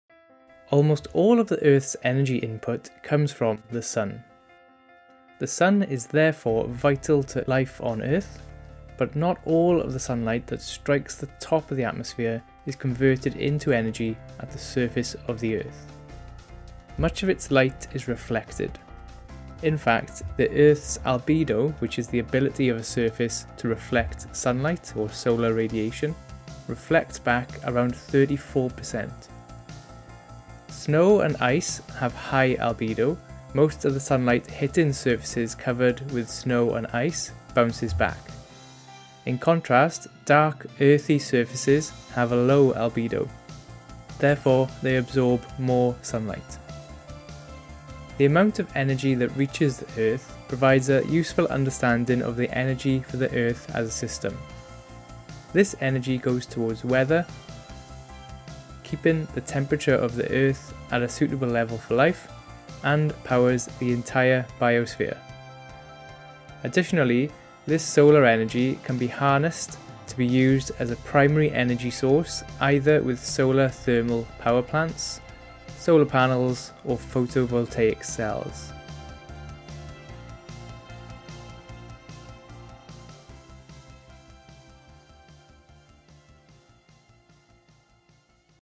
Energy From The Sun
Almost all of the Earth's energy input comes from the sun. The Sun is therefore vital to life on Earth, but not all of the sunlight that strikes the top of the atmosphere is converted into energy at the surface of the Earth.